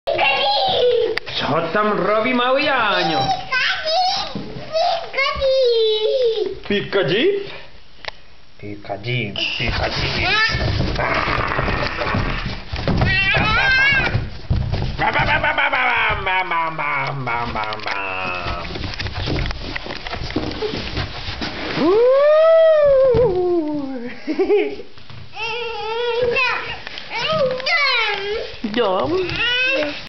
2009.03 Janio_w_kartonie
Janio bawi się w pudle